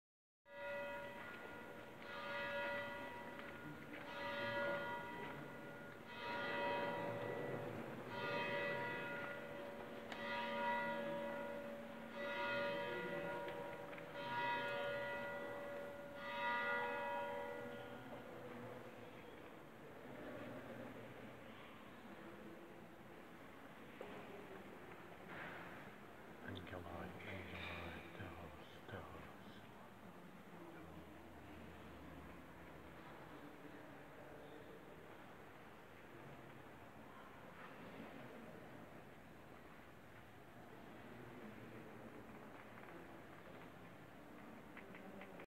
Mensagens angélicas no convento do Carmo, em Lisboa
Visita rápida ao convento do Carmo, com gravação espontânea, aquando de uma mesa redonda que moderei sobre o livro Vicente, projecto da Travessa da Ermida e do Mário Caeiro. 2018